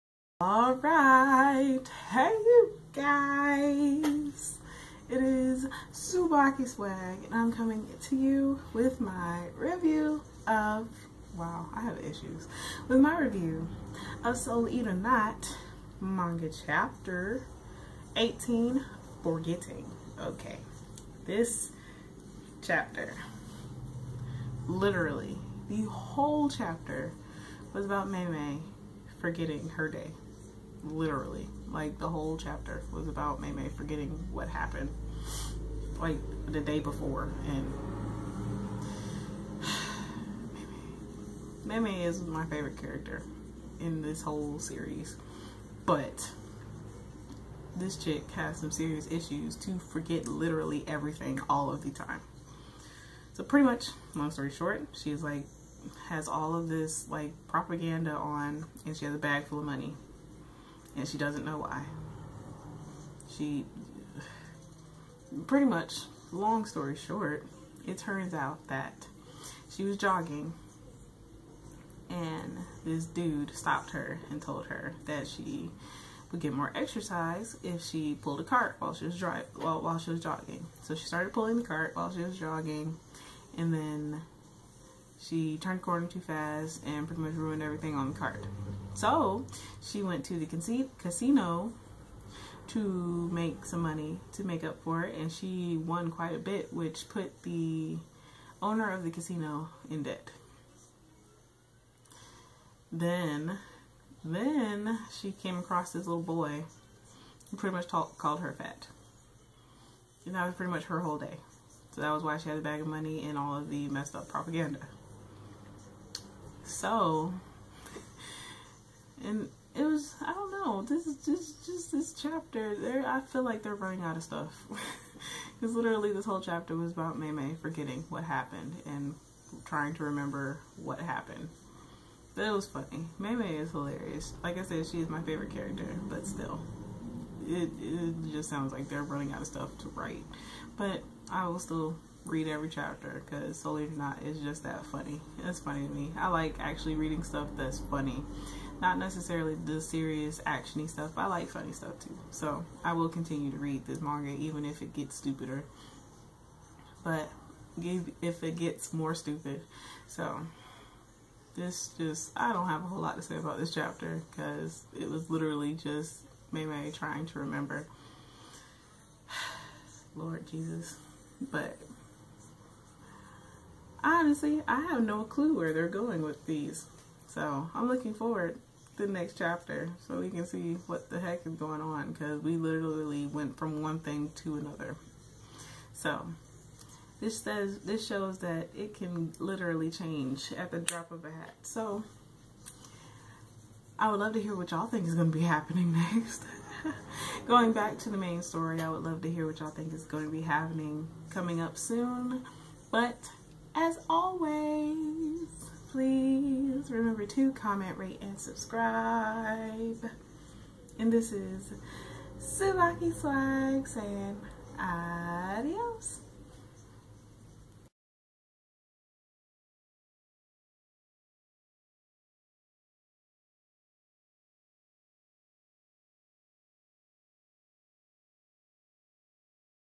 Soul Eater Not! Manga Chapter 18 Review- Really Meme?! Forgetful Much?!
This chapter just made me lose all hope in humanity but i will still read it. smdh lol

Kik:
TsubakiSwagg